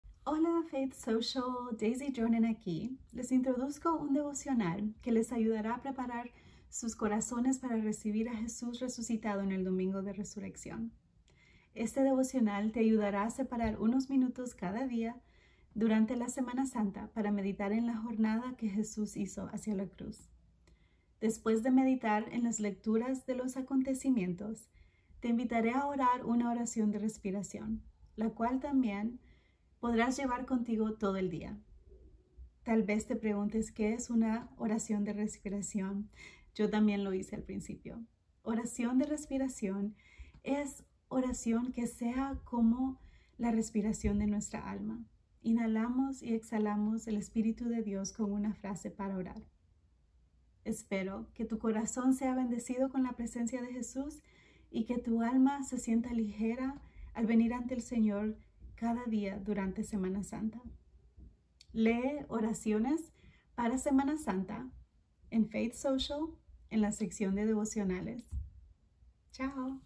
Devocional para Semana Santa
Prepara tu corazón para el domingo de resurrección con este devocional de 5 días de Daisy Dronen